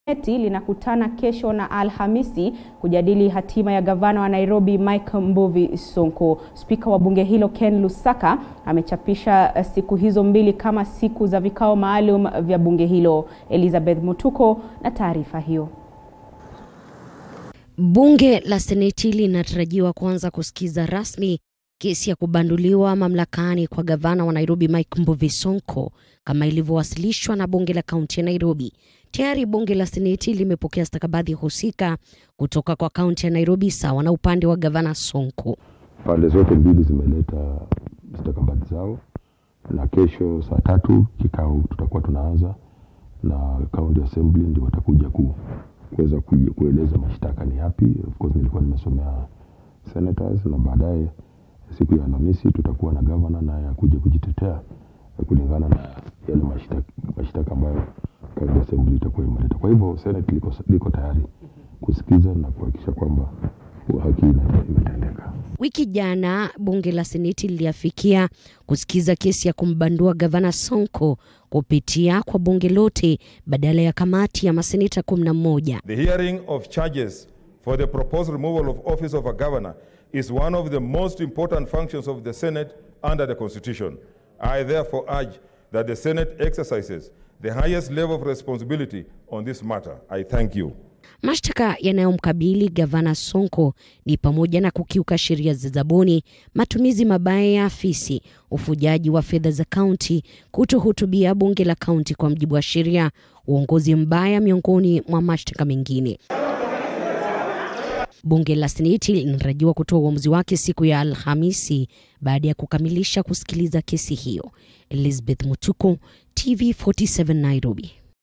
Bunge la Seneti kujadili hatma ya Sonko Alhamisi
HATIMA YA GAVANA SONKO
Bunge la Seneti linakutana kesho na Alhamisi kujadili hatima ya gavana wa Nairobi Mike Sonko, Spika wa bunge hilo Ken Lusaka amechapisha siku hizo mbili kama siku za vikao maalum vya bunge la Seneti.